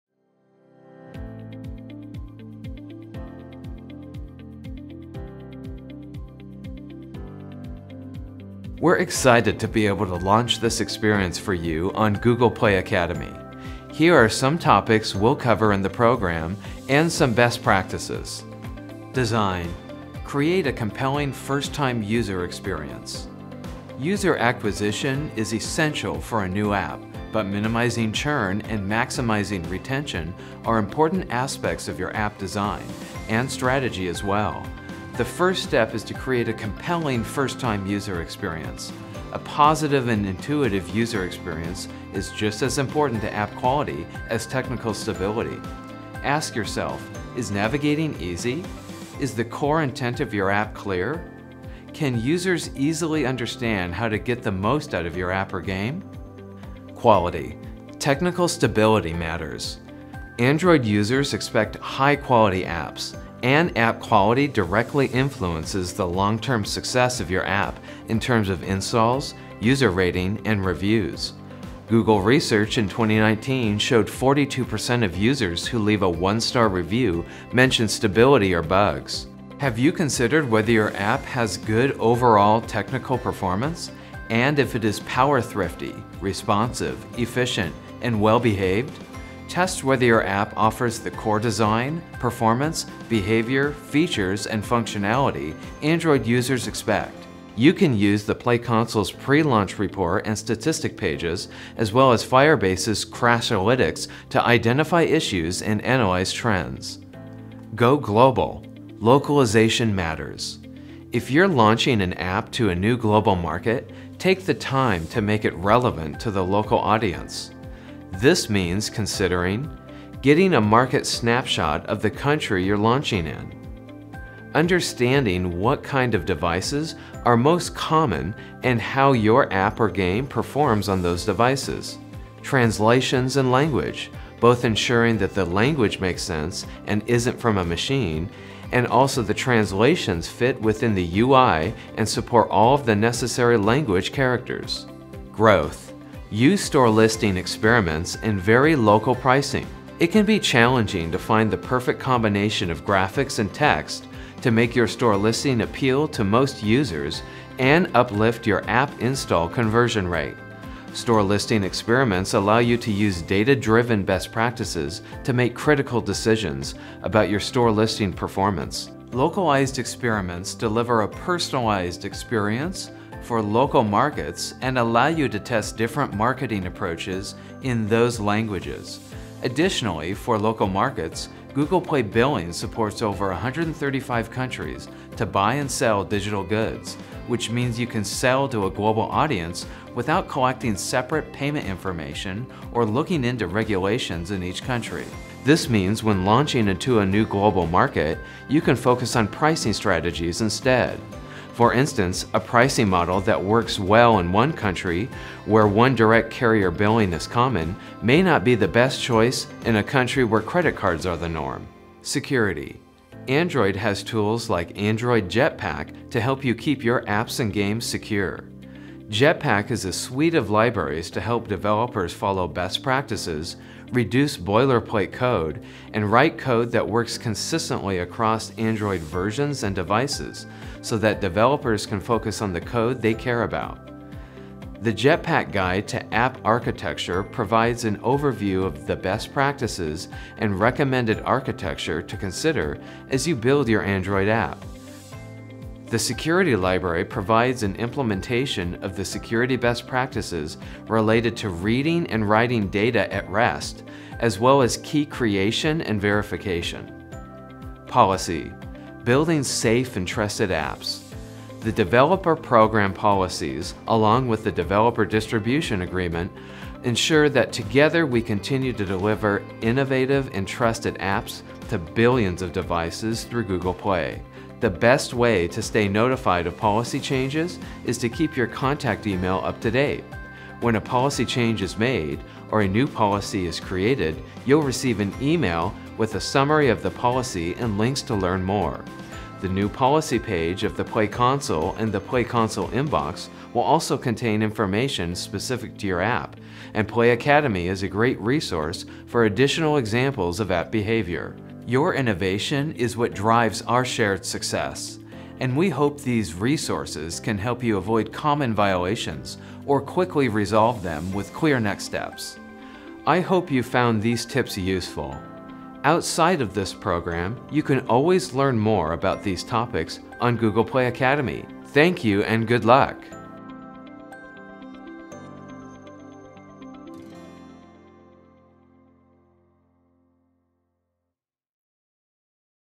Appscale Academy by Google Play and MeitY Startup Hub
Upskill on Google Play Academy!

Appscale Academy is a growth and development program by the Ministry of Electronics and Information Technology (MeitY) Startup Hub and Google Play to help early to mid-stage startups across India build high-quality apps for the world.

We know the impact apps have in enabling solutions as more and more users come online. Whether you have built a game to delight, a healthcare app trying to reach those in need, a fintech app targeting lesser penetrated geographies, or an e-commerce delivery app, we bring to you a program that will power your next stage of growth.